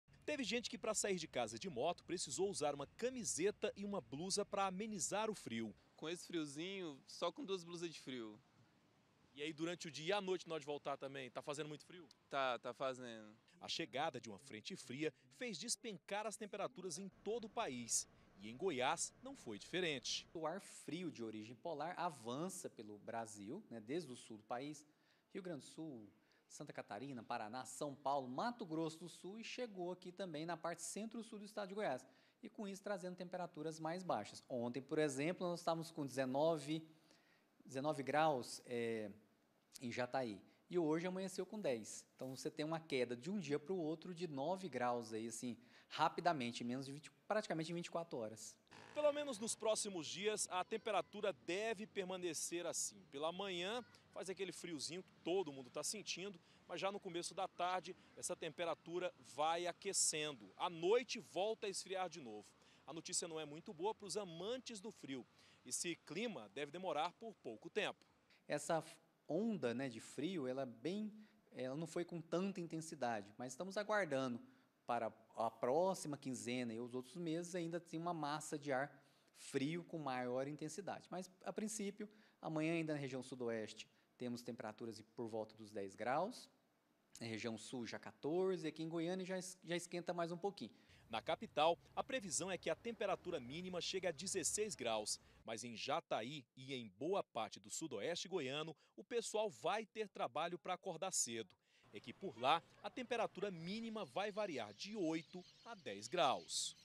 PRIMEIRA DO ANO: FRENTE FRIA CHEGA E DERRUBA TEMPERATURA EM GOIÁS
QUEM CURTE UM TEMPO MAIS FRIO, UM CASACO, UMA COBERTA DE NOITE,  COMEMOROU A PRIMEIRA FRENTE FRIA DO ANO.